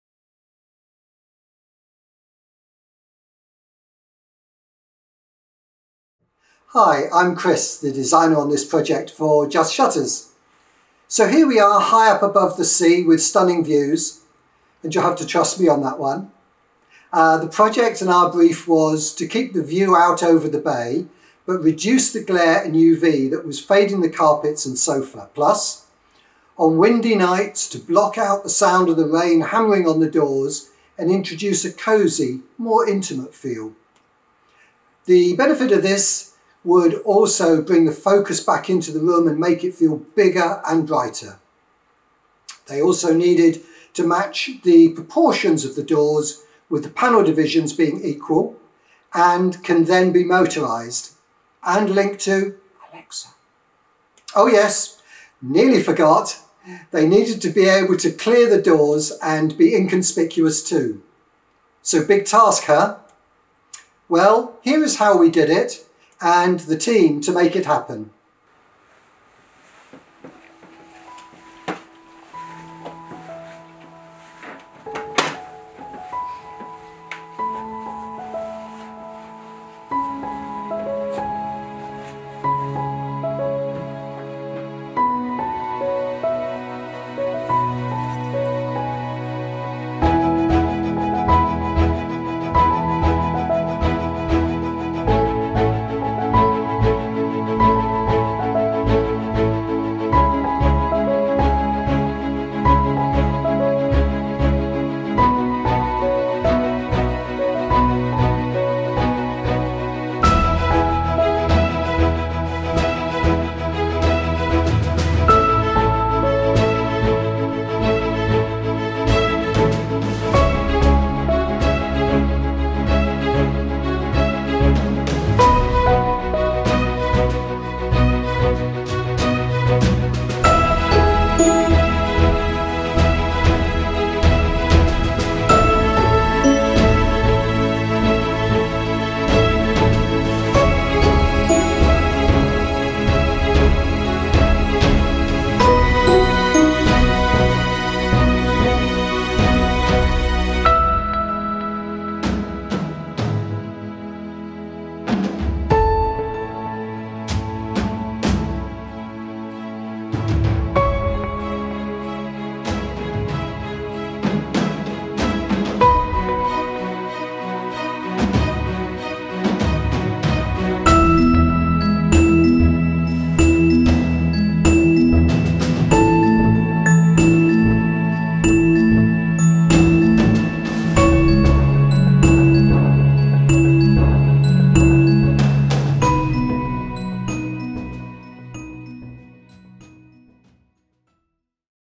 Just Shutters Install & Demonstration with remote
A time-lapse video of a shutter installation with handheld remote control.